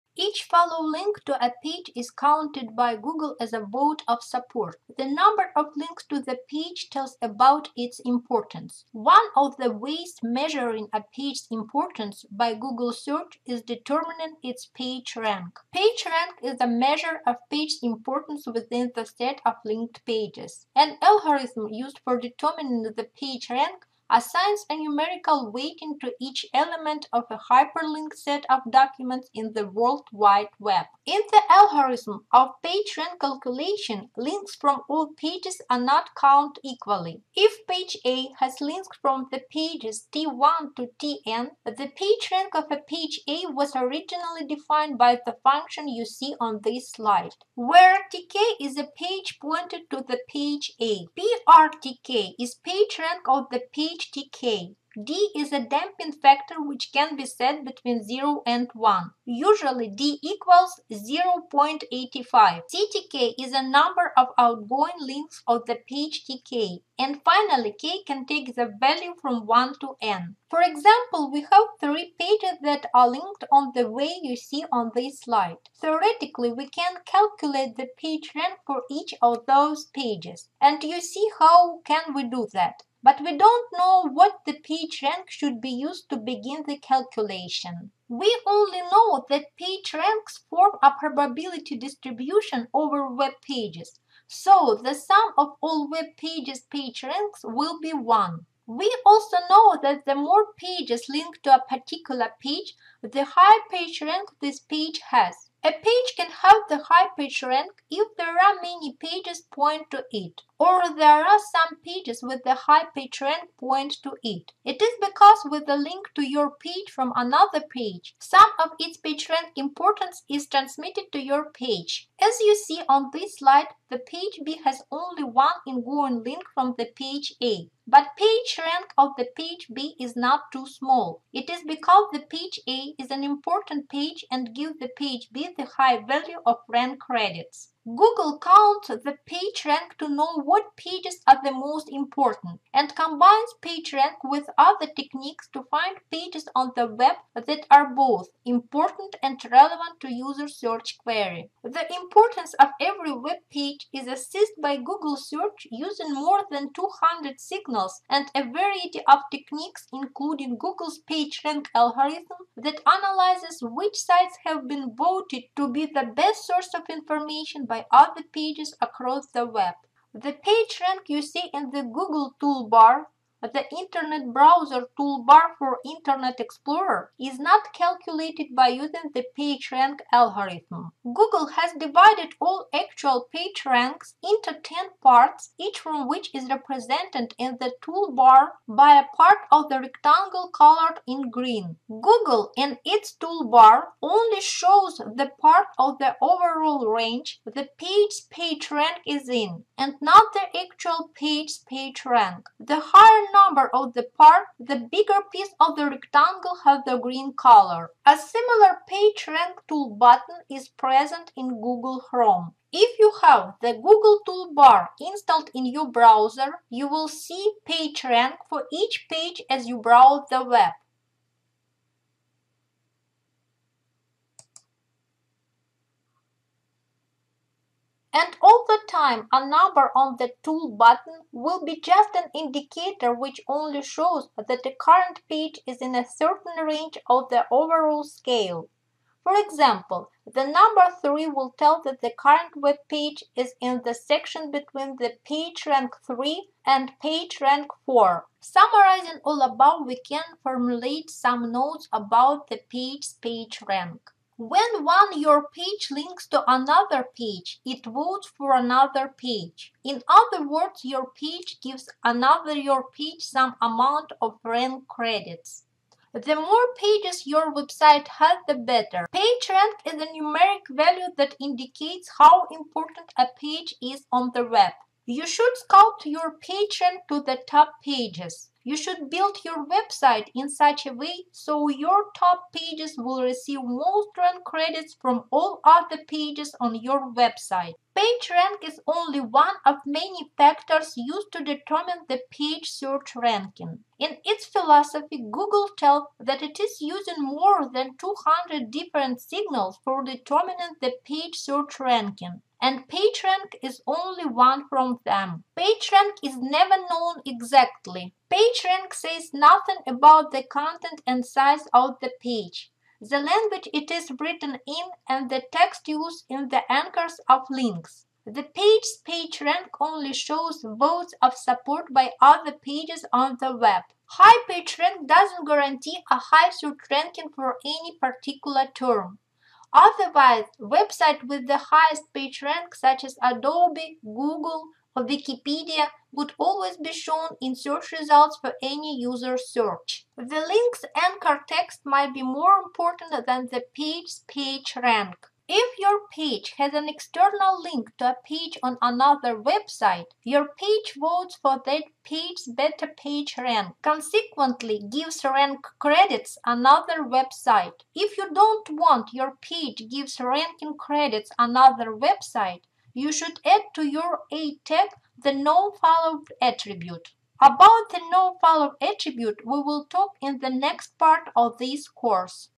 SEO for Beginners – Part 10: PageRank
Each “follow” link to a page is counted by Google as a vote of support. PageRank is a measure of page’s importance within the set of linked pages.